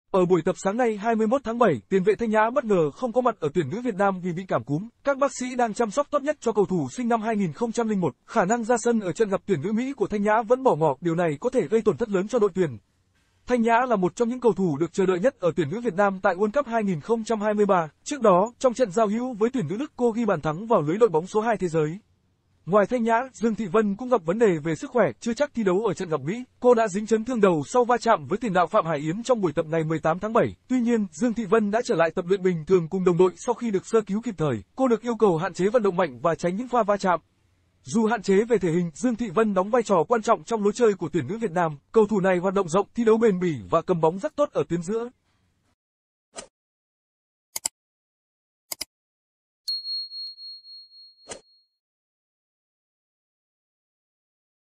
LO LẮNG VÌ SỨC KHỎE CỦA THANH NHÃ VÀ DƯƠNG THỊ VÂN
Ở buổi tập sáng nay (21/7), tiền vệ Thanh Nhã bất ngờ không có mặt ở tuyển nữ Việt Nam vì bị cảm cúm. Các bác sĩ đang chăm sóc tốt nhất cho cầu thủ sinh năm 2001. Khả năng ra sân ở trận gặp tuyển nữ Mỹ của Thanh Nhã vẫn bỏ ngỏ, điều này có thể gây tổn thất lớn cho đội tuyển.